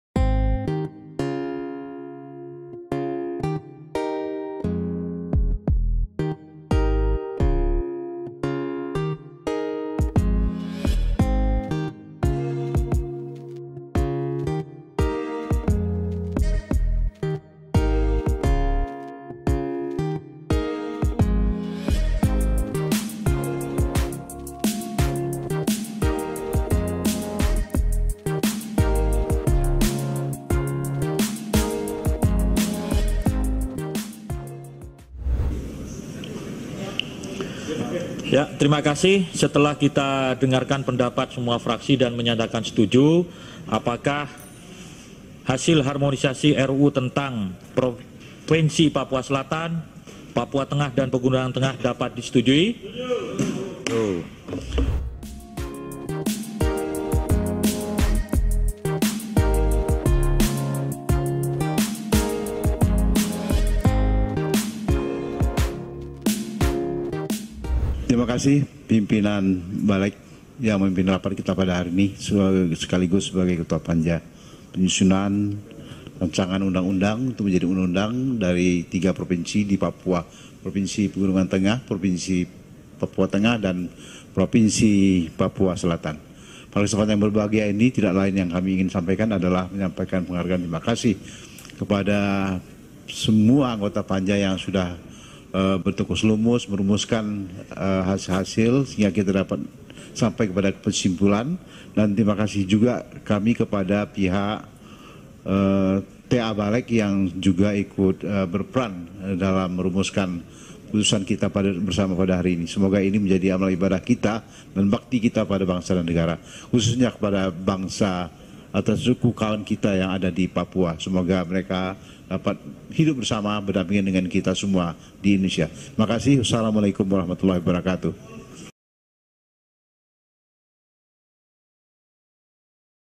Indonesia Akan Punya 3 Provinsi Baru di Papua, Apa Saja?
JAKARTA, KOMPAS.TV  Indonesia akan mempunyai 3 Provinsi baru yakni Provinsi Papua Selatan (Ha Anim), Provinsi Papua Tengah (Meepago) dan Provinsi Papua Pegunungan Tengah (Lapago).

Rencana ini diatur dapal Rancangan Undang-Undang tentang 3 provinsi baru tersebut, dan telah disahkan oleh Badan Legislasi DPR RI dalam rapat pleno yang digelar pada Rabu (6/4)

"Setelah kita mendengarkan pendapat semua fraksi dan menyatakan setuju. Apakah hasil harmonisasi RUU tentang Provinsi Papua Selatan, Papua Tengah dan Papua Pegunungan Tengah dapat disetujui?" tanya Wakil Ketua Baleg DPR Achmad Baidowi dalam rapat.

Semua fraksi di Baleg menyatakan setuju terhadap RUU tentang tiga provinsi tersebut.

"Setuju," jawab para peserta sidang.

Provinsi Papua Selatan akan memiliki Ibu Kota Merauke, Provinsi Papua Tengah dengan Ibu Kota Timika, Kabupaten Mimika dan Provinsi Papua Pegunungan Tengah dengan Ibu Kota Wamena di Kabupaten Jayawijaya